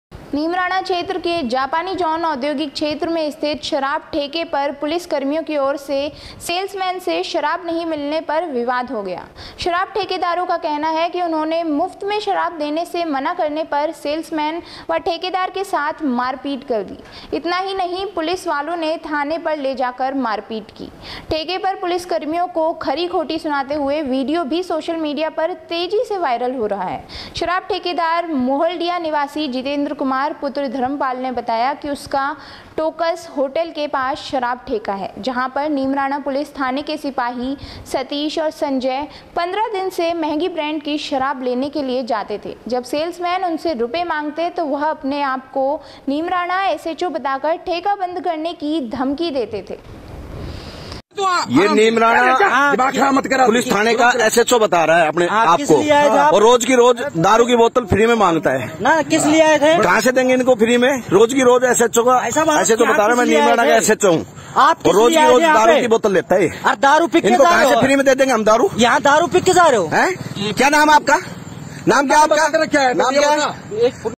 alwar news//फ्री में शराब नहीं देने पर पुलिसकर्मियों और सेल्समैन में हुआ विवाद//22/7/2021
नीमराणा क्षेत्र के जापानी जॉन औधोगिक क्षेत्र में स्थित शराब ठेके पर पुलिसकर्मियों की ओर से सेल्समैन से शराब नही मिलने पर विवाद हो गया।शराब ठेकेदारों का कहना है की उन्होंने मुफ्त में शराब देने से मना करने पर सेल्समैन व ठेकेदार के साथ मारपीट कर दी।इतना ही नहीं,पुलिस वालों ने थाने पर ले जाकर मारपीट की।ठेके पर पुलिसकर्मियों को खरी-खोटी सुनाते हुए वीडियो भी सोशल मीडिया पर तेजी से वायरल हो रहा है।
शराब ठेकेदार मोहलड़िया निवासी जितेंद्र कुमार पुत्र धर्मपाल ने बताया कि उसका टोकस होटल के पास शराब ठेका है, जहां पर नीमराणा पुलिस थाने के सिपाही सतीश और संजय 15 दिन से महंगी ब्रांड की शराब लेने के लिए जाते थे. जब सेल्समैन उनसे रुपये मांगते तो वह अपने आपको नीमराणा एसएचओ बताकर ठेका बंद करने की धमकी देते थे।